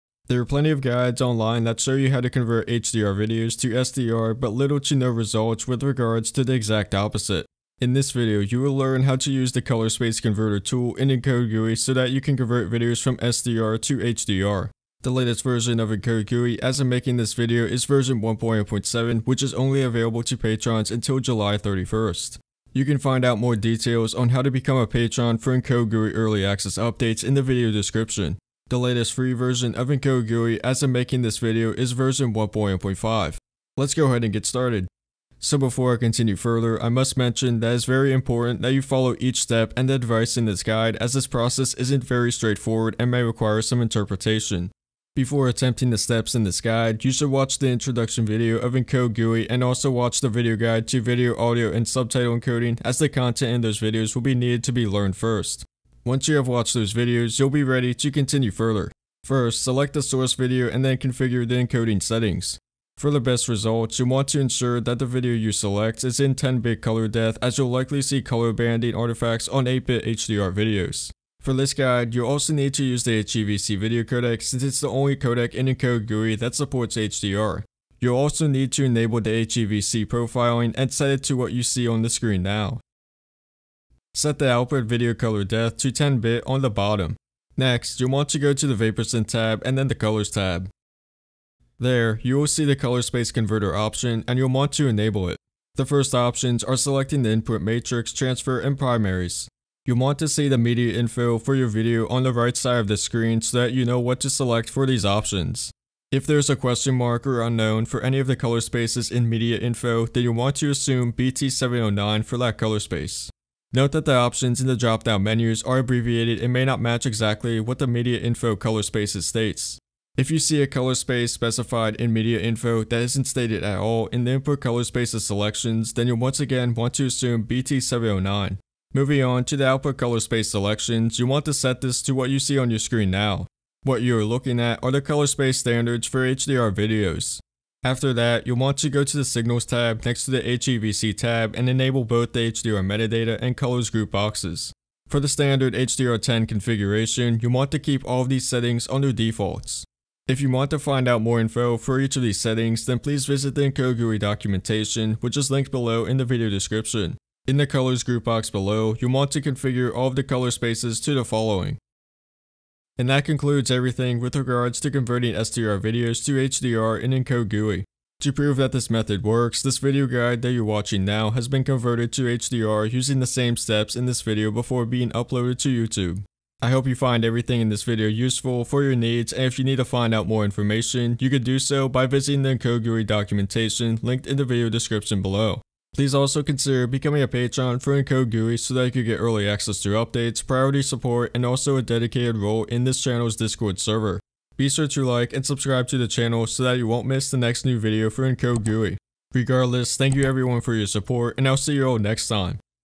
[EncodeGUI Official Video Tutorial] How to Convert SDR Videos to HDR | Color Space Converter Guide
This is part 5 of 6 of the EncodeGUI official video tutorial series. In this video, you will learn how to use the Color Space Converter tools featured in EncodeGUI so that you can convert SDR videos to HDR. In order to get the full HDR experience, your video must be in 10 bit colors and the monitor that you view the video on must feature HDR capabilities with 1000 nits of screen brightness.